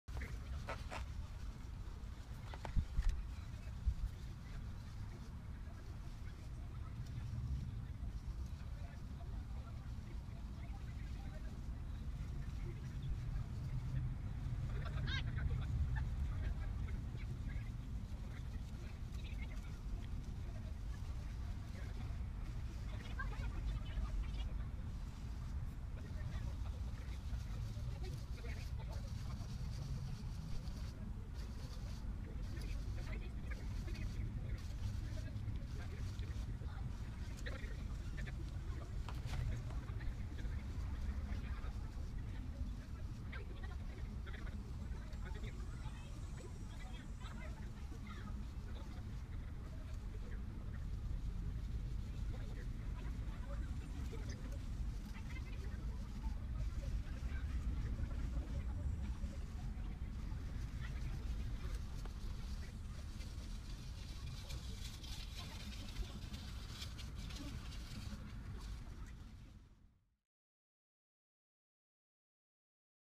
Bowie first autonomous navigation for 277 seconds!
Auton navigation using AR marker detection. No human assists! For 277 seconds. This is a good first step towards getting the robot to clean a section of the beach on its own